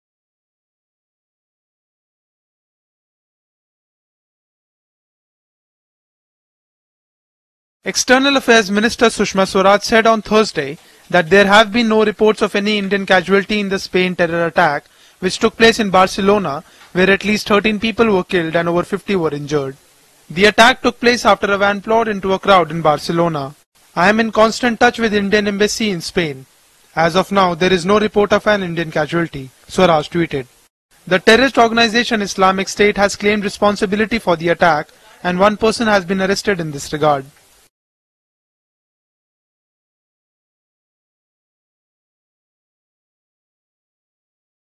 No report of Indian casualty in Barcelona terror attack, confirms Sushma Swaraj
External Affairs Minister Sushma Swaraj said on Thursday, that there have been no reports of any Indian casualty in the Spain terror attack which took place in Barcelona, where at least 13 people were killed and over 50 were injured. The attacked took place after a van plowed into the crowd in Barcelona.  &ldquo;I am in constant touch with Indian Embassy in Spain @IndiainSpain. As of now, there is no report of an Indian casualty.&rdquo; Swaraj tweeted. The terrorist organisation Islamic State (ISIS) has claimed responsibility for the attack and one person has been arrested in this regards